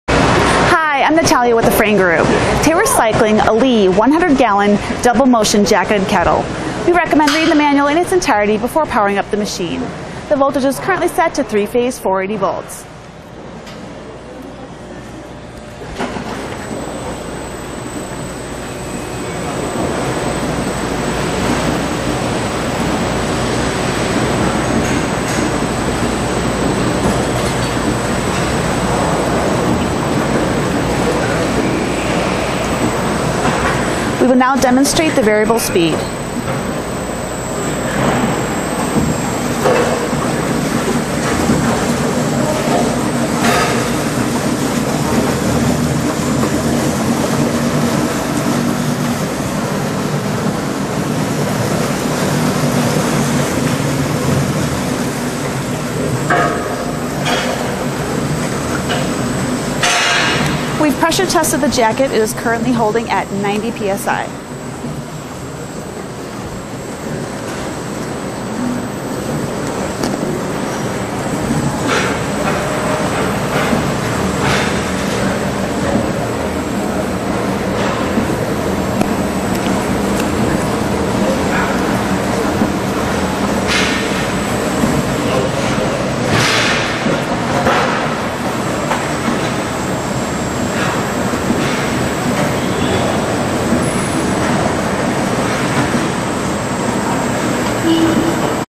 Lee 316 Stainless Steel Jacketed Kettle Demonstration
Lee, Model 100 D9MS, 316 stainless steel jacketed kettle. Vessel Dimensions: 36" ID x 30" Deep mixing bowl with a 12" straight wall. Equipped with bridge mounted, variable speed scrape surface agitator, flip top covers and (1) flush mount sensor. Dish bottom with 2½" OD / 2" ID  tri-clamp style center bottom discharge with ball valve and 22" floor clearence. Mounted on (3) stainless steel tubular leg frame.

Jacket Fittings: (2) 1" NPT

MAWP: 90 PSI @ 332° F
MDMT:  -20°F @ 90 PSI
Nat'l Brd: 11781